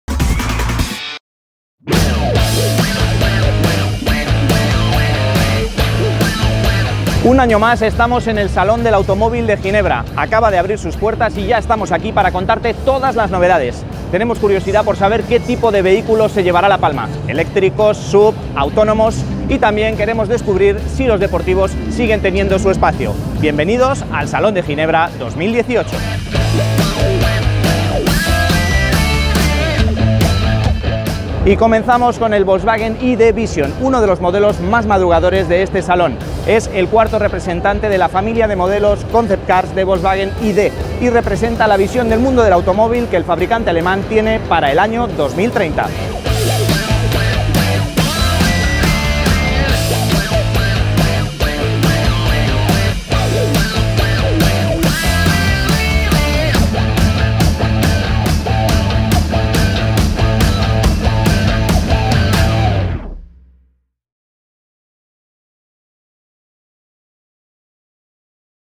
¡Arranca el Salón de Ginebra 2018!
Ya se ha puesto en marcha el Salón de Ginebra 2018, que ha abierto sus puertas con muchísimas novedades que os iremos contando. Es sin duda, una de las citas importantes del año en el mundo del automóvil y donde todas las marcas dan el do de pecho para mostrar sus últimos productos. La edición 88º del Geneve Autoshow, que se celebra este año, tendrá lugar entre los días 8 y 18 de marzo, siendo reservados los días 6 y 7 para la prensa y medios especializados, y contará con hasta 180 stands de diferentes fabricantes y marcas afines a la industria del automóvil que darán a conocer las últimas novedades del sector.

El modelo más madrugador ha sido el Volkswagen I.D. Vizzion Concept. La marca alemana sigue sumida en su visión de futuro, adelantando en cada cita importante del sector uno de los modelos que espera poner en circulación a medio plazo.